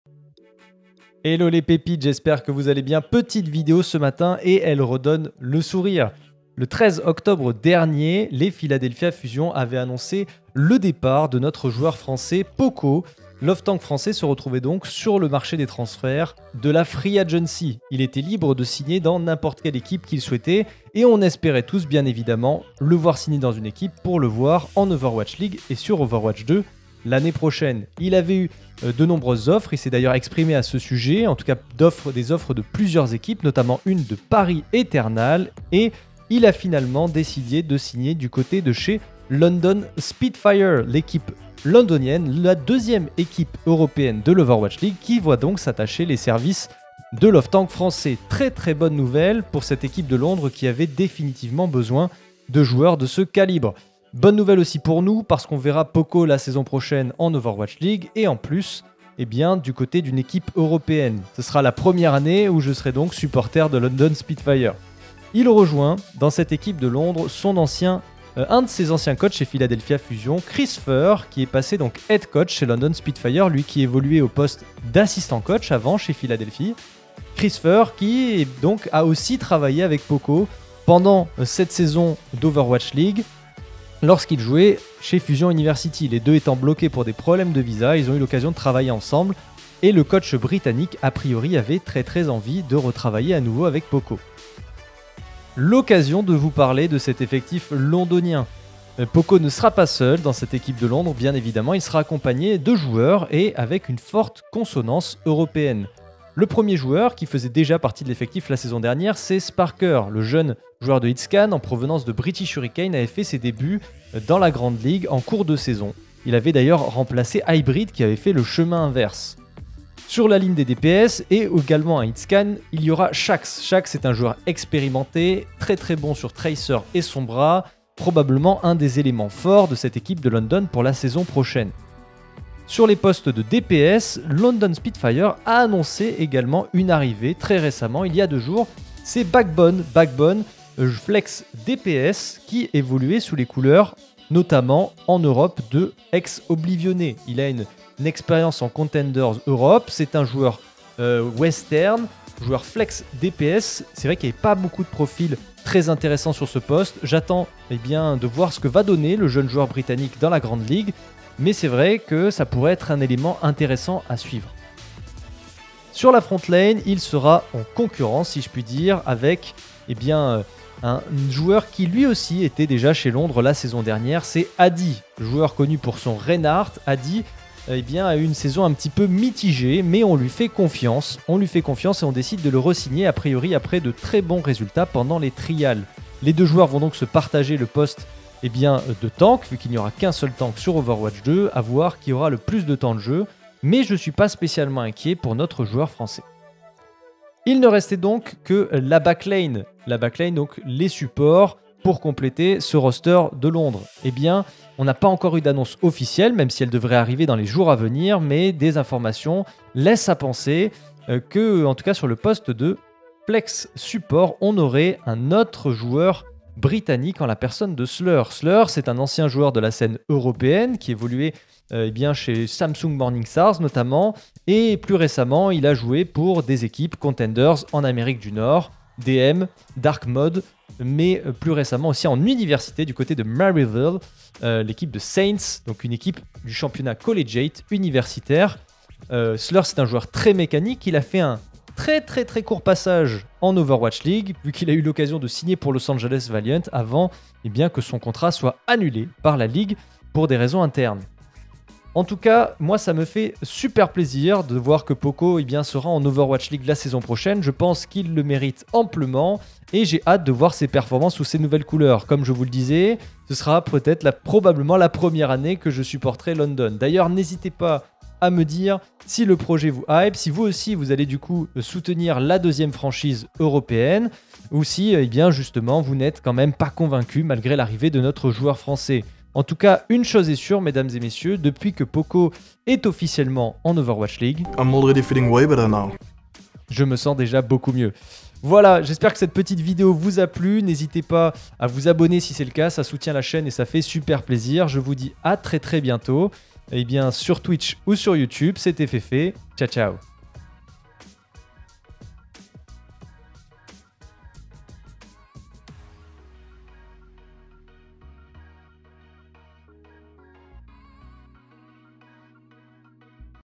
POKO CHEZ LONDON SPITFIRE EN 2022 !
Hier London Spitfire a annoncé la signature de l'offtank français au sein de son effectif pour la saison prochaine !